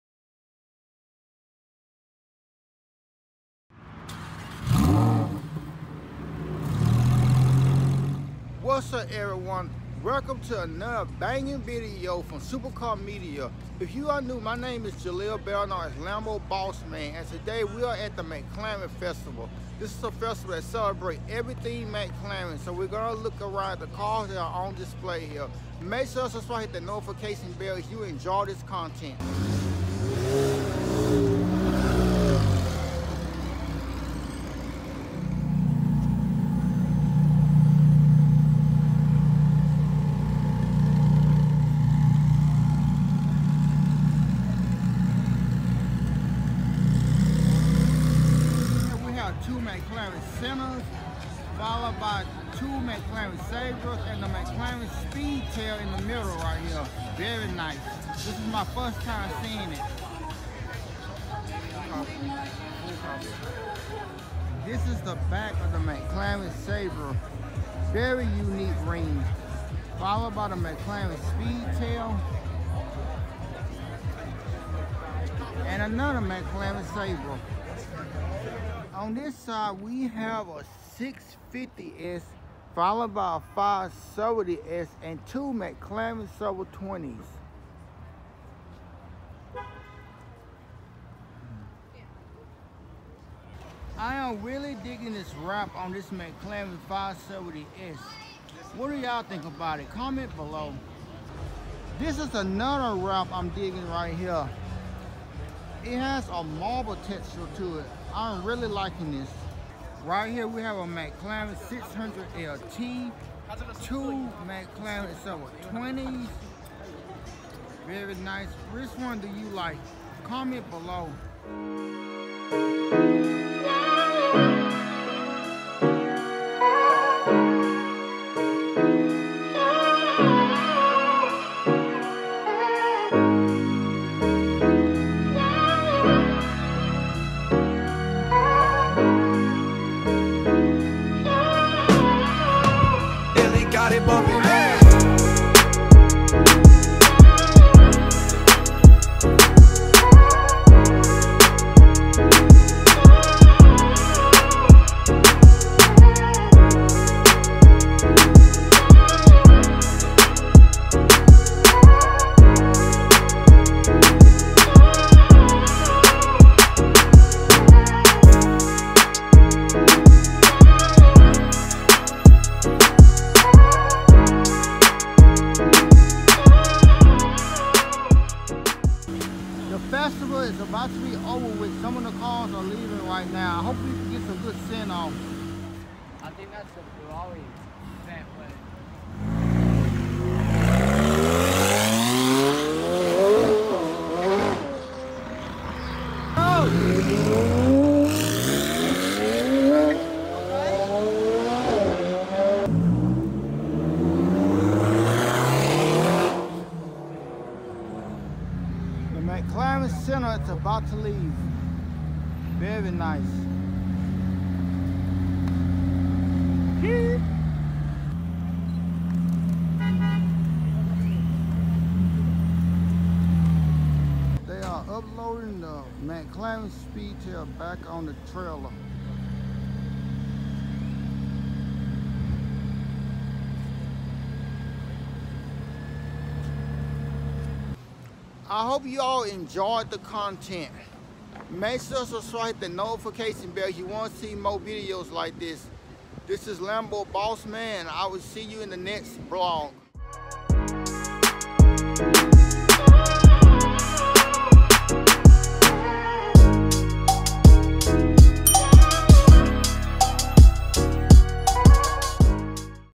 Mclaren Sabre steals the show at the McLaren festival! (Sabre, Senna, SpeedTail ,720s)
I attended the McLaren festival which is the first of its kind at the River Oaks district in Houston Tx. The festival was hosted by McLaren Houston and featured every McLaren model you can think of included the SpeedTail, Senna and Sabre. I hope you all enjoy the content!!